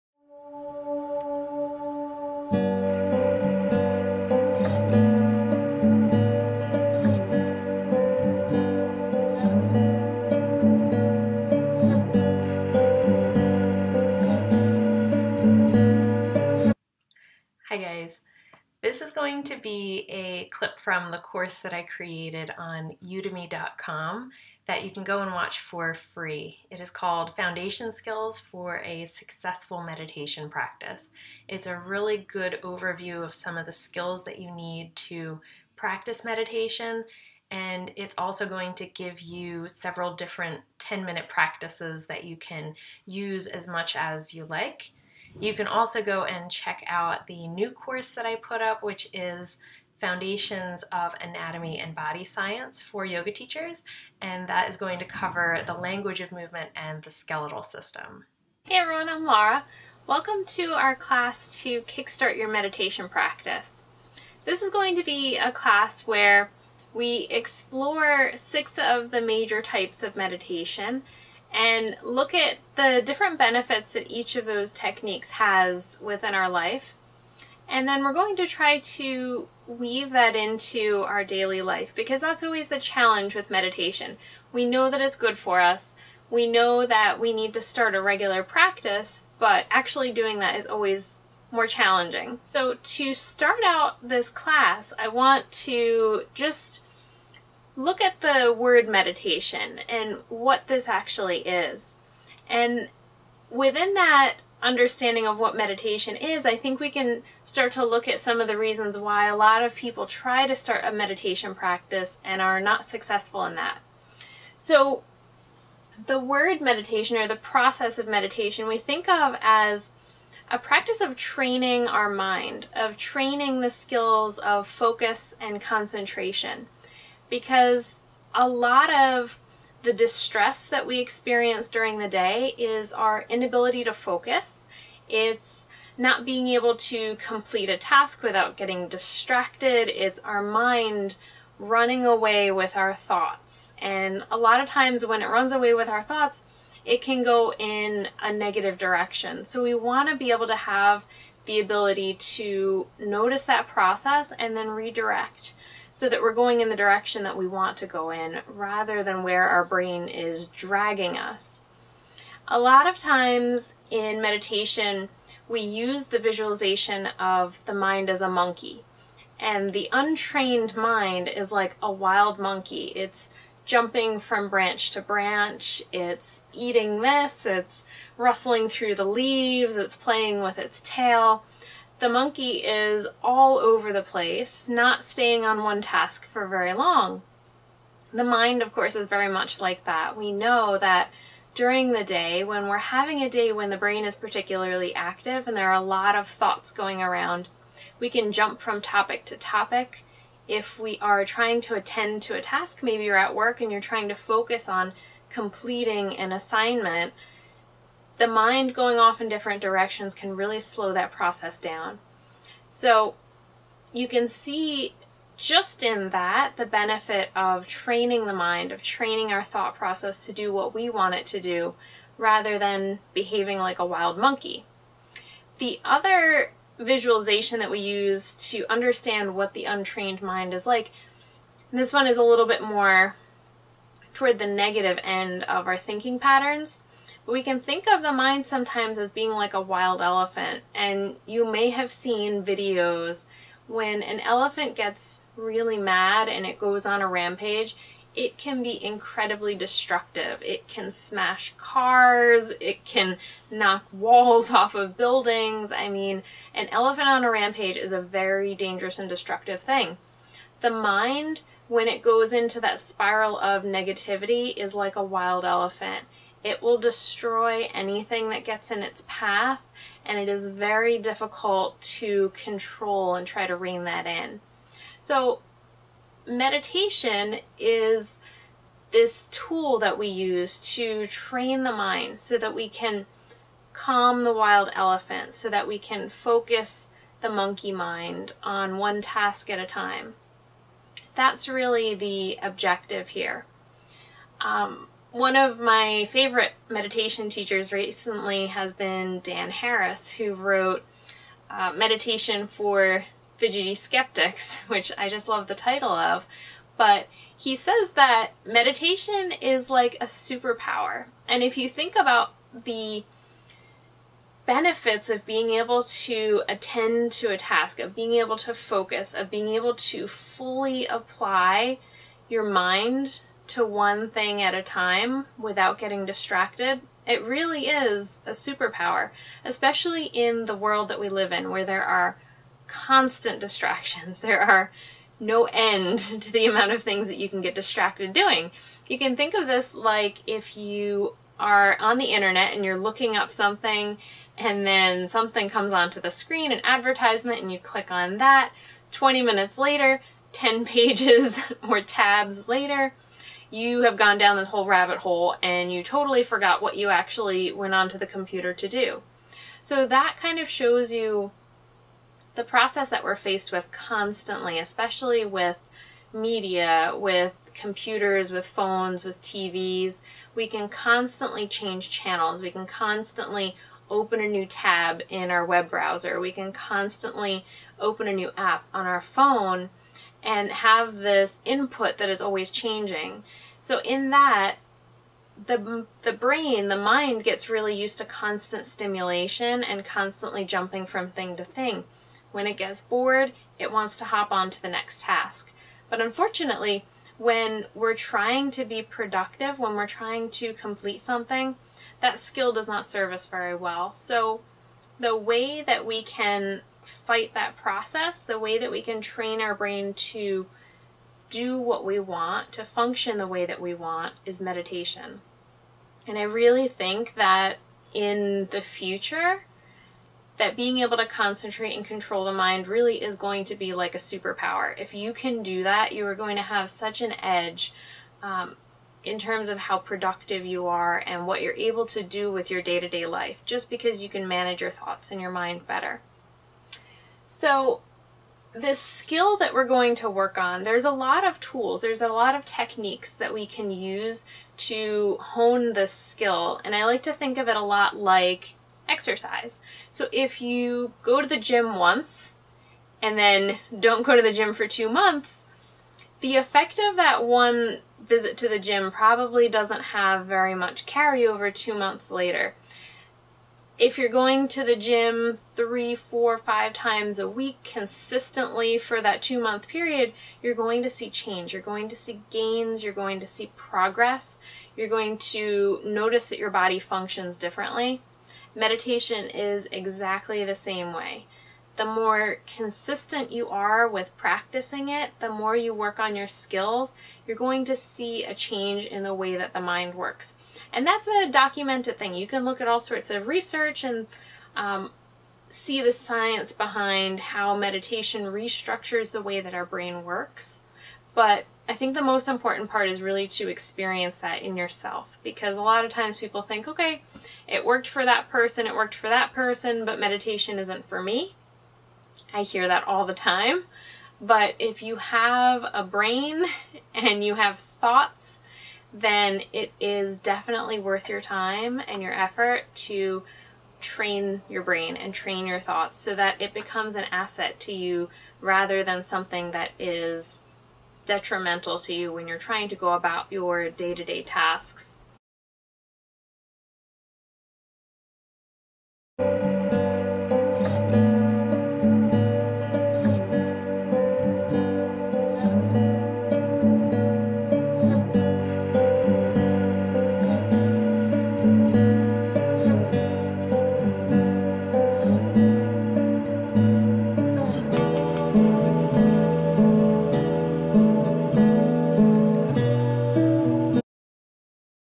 What is Meditation Vs. Concentration: Dharana & Dhyana in the 8 Limbs of Yoga- LauraGyoga
This video is a lecture from my free online course hosted by Udemy.com: "Foundations Skills For a Successful Meditation Practice". We start out by discussing the difference between Concentration & Meditation. Video by Laura Goellner OTR, E-RYT, LMT, CLT.